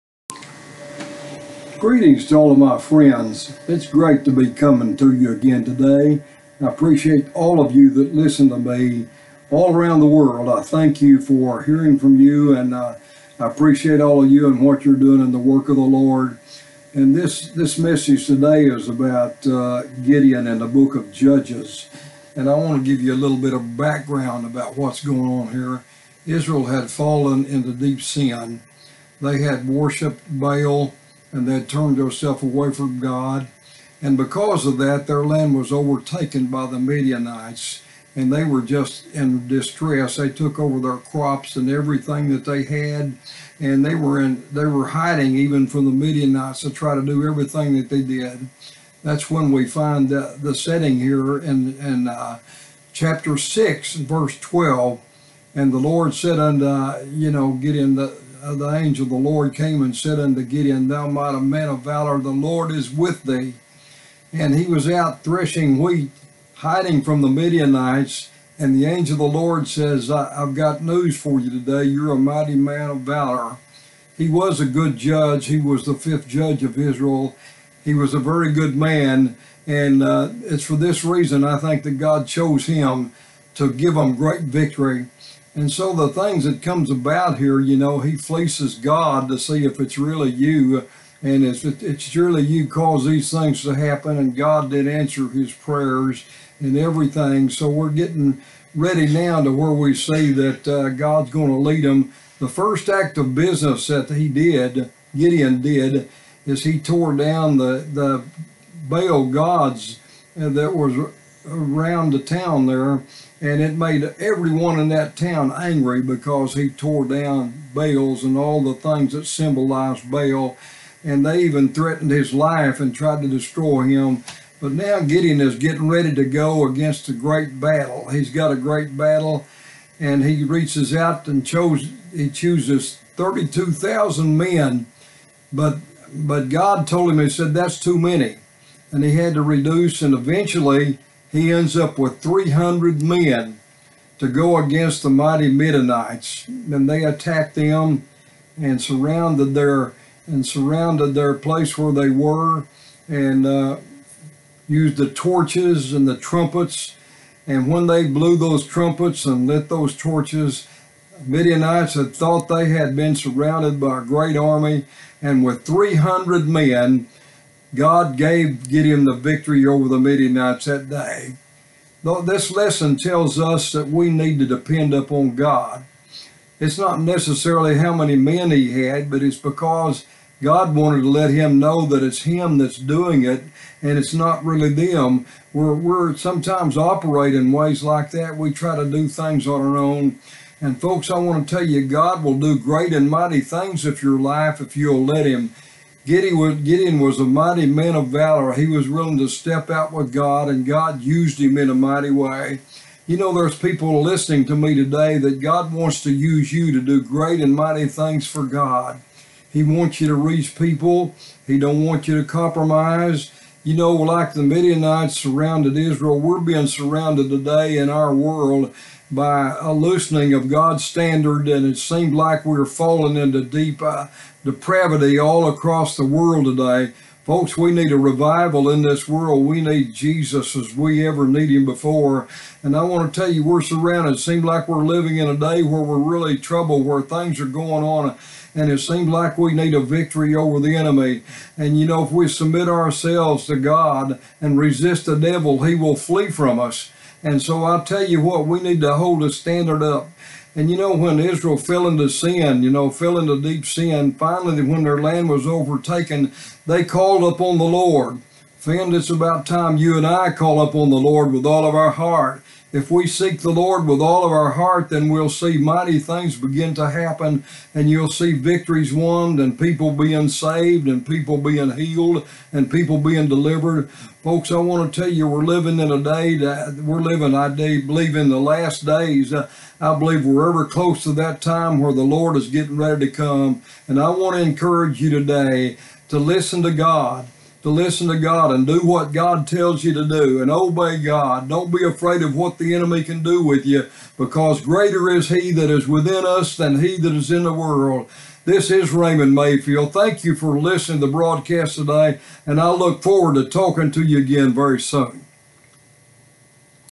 GIDEON, MAN OF VALOR!
In this video, from chapter 6 of the book of Judges, Gideon is called upon to save the people of Israel by leading them against the Midianites. Israel had done evil in the sight of the Lord, and when their land was overtaken, they called on the Lord. Enter Gideon, who is called upon to lead the people to victory.